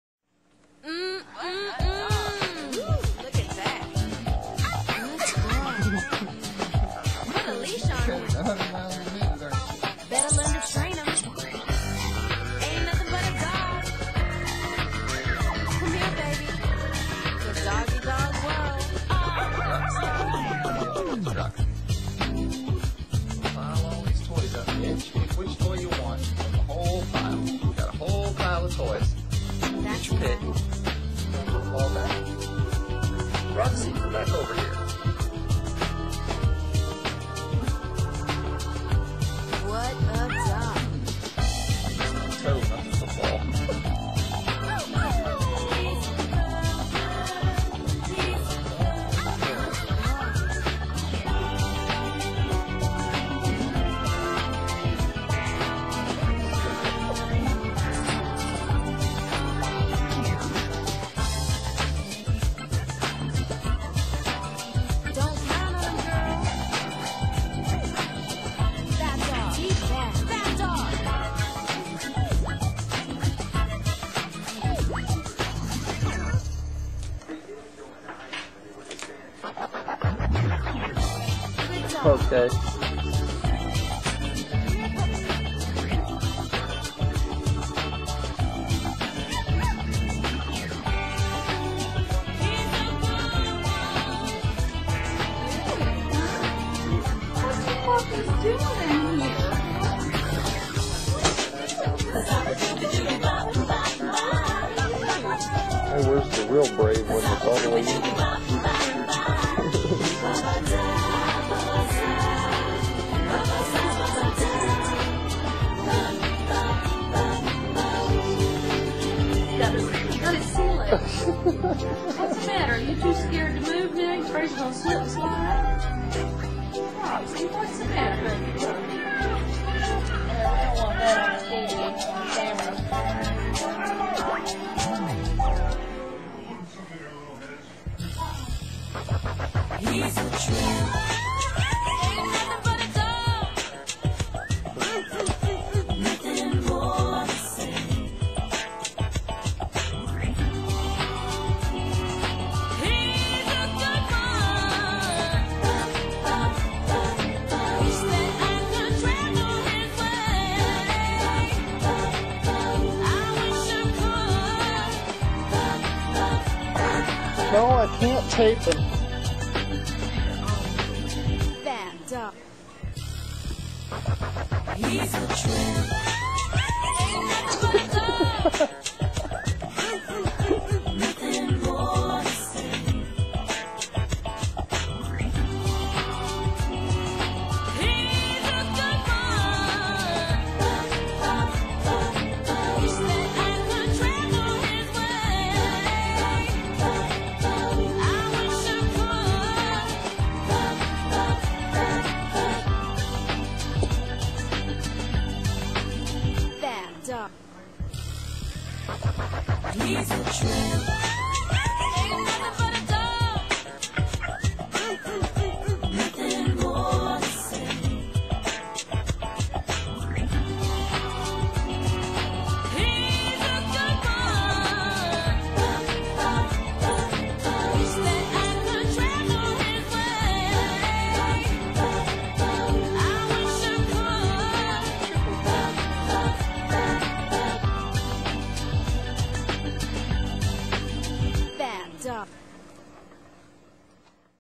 More Puppy Play 07-6wks old now.
The puppies are getting even more outgoing and playful. The cutest litter of puppies in the whole world I'm sure!